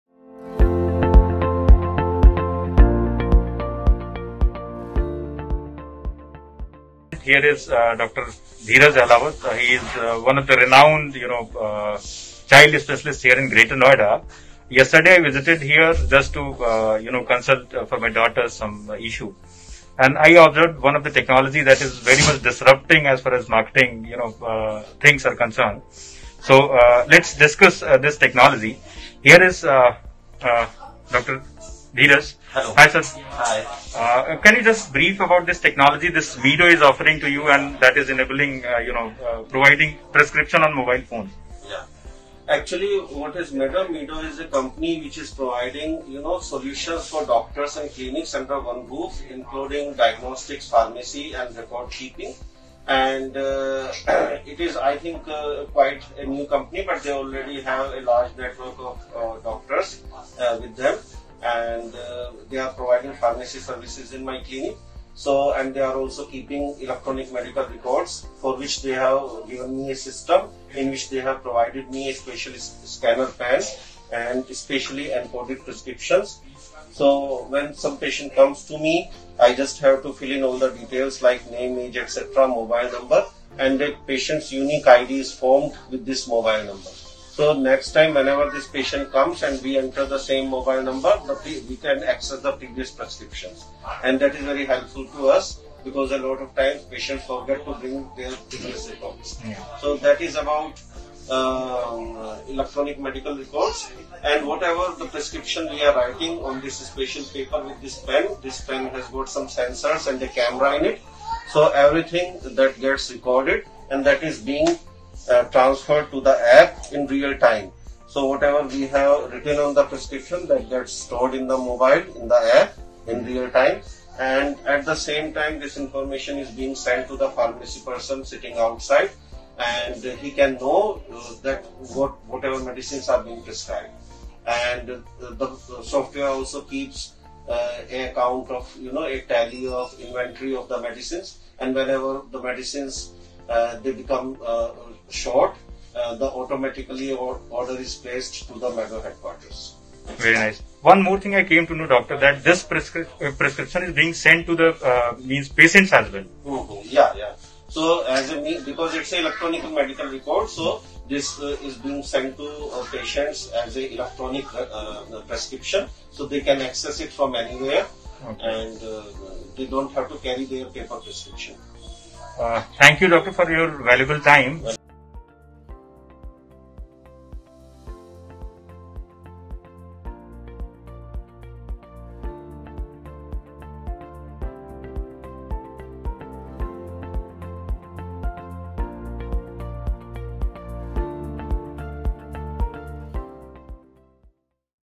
Patients React to Revolutionary Technology behind their digitizedprescriptions Meddo-Doxper
Meddo-doxper helps doctors provide the best patient care possible by digitizing healthcare and driving productivity to improve outcomes for patients, providers, and health systems around the world.